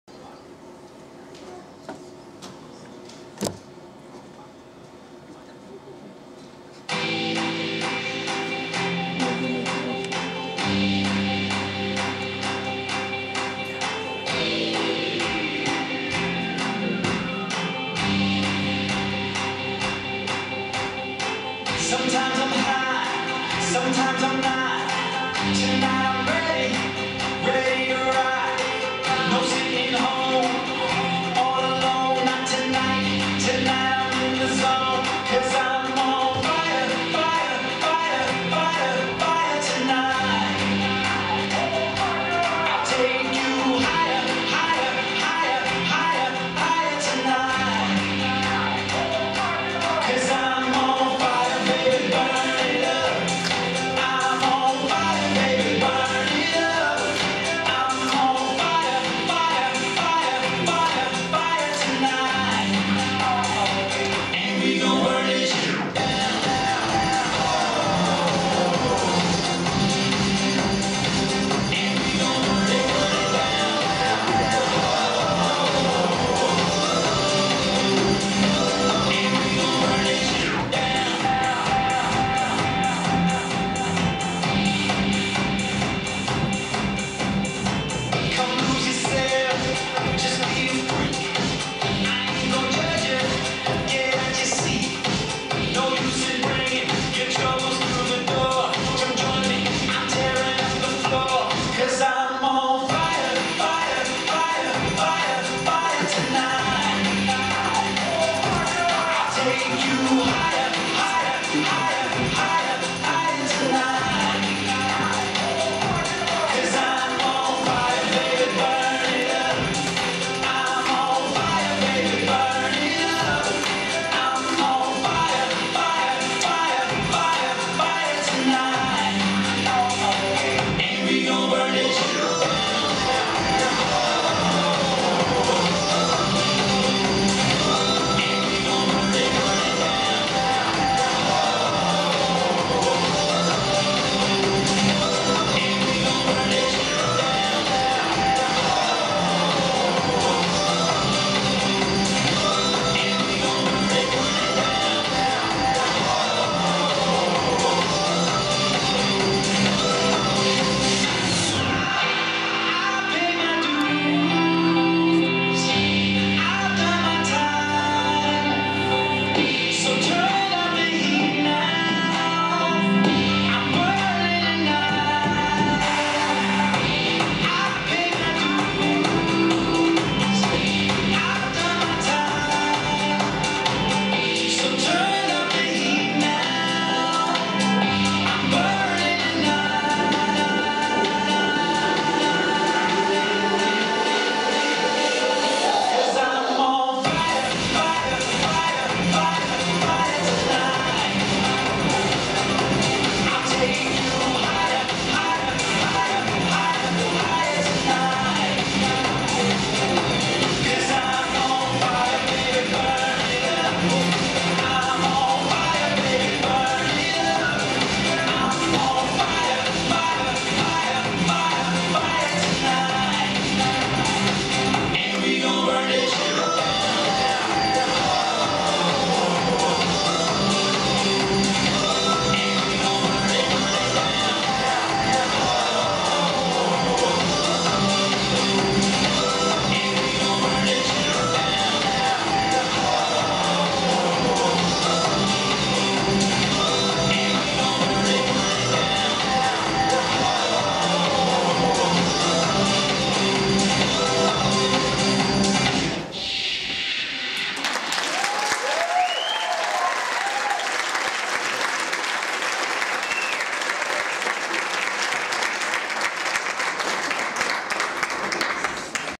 Fire
Highland Dance Recital 2012
Choreographed by Elise Diewart